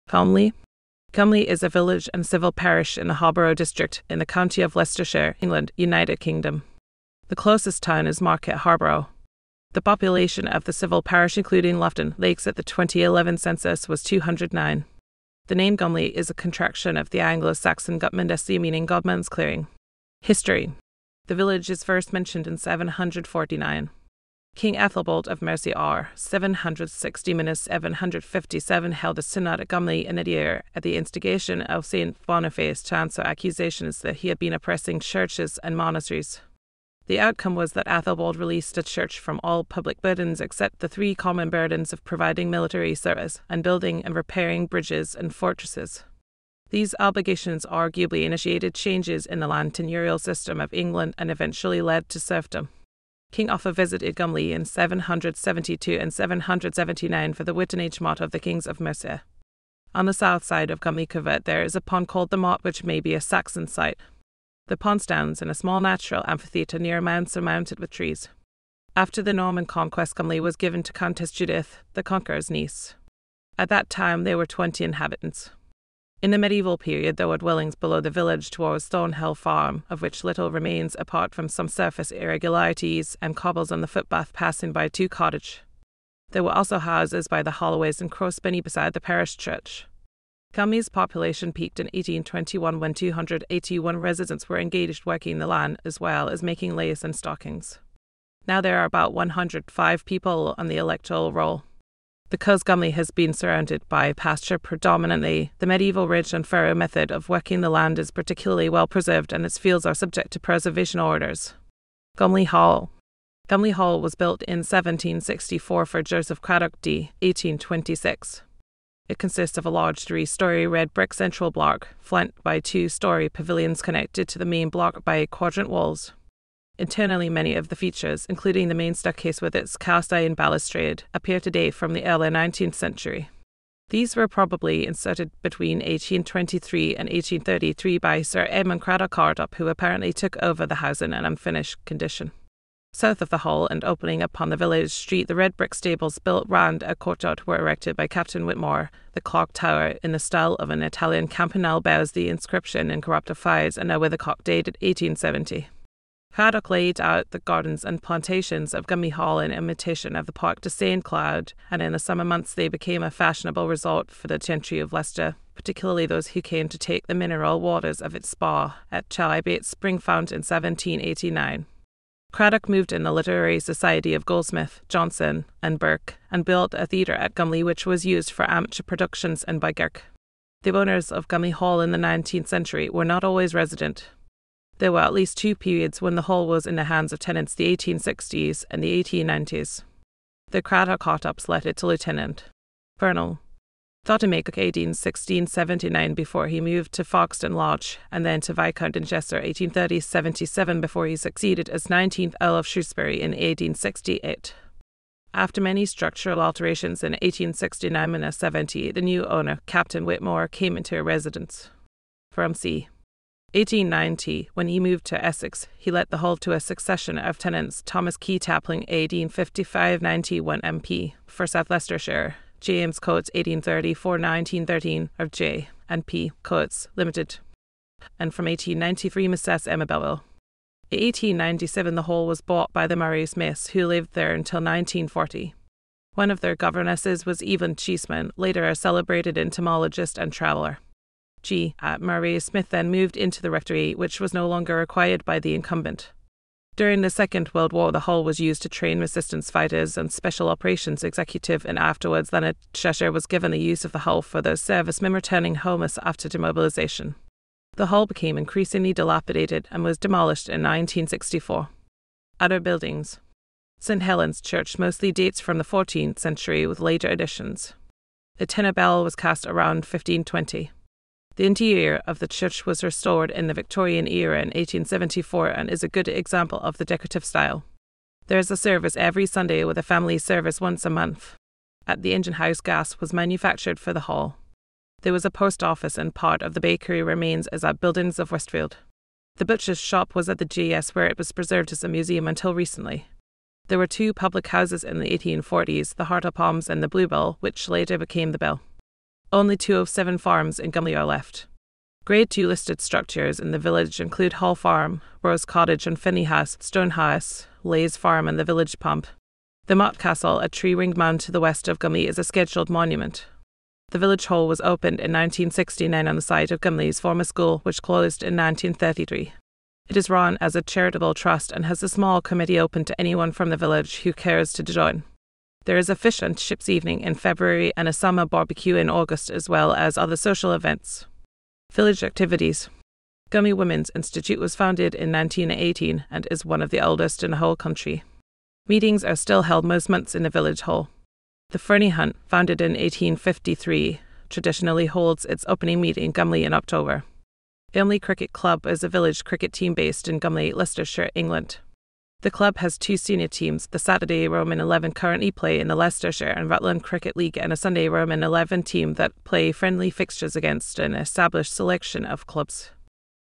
Gumley
Gumley is a village and civil parish in the Harborough district, in the county of Leicestershire, England, United Kingdom. The closest town is Market Harborough. The population of the civil parish (including Laughton, Leics) at the 2011 census was 209.
The name Gumley is a contraction of the Anglo-Saxon "Gutmundesleah" – meaning Godmund's clearing.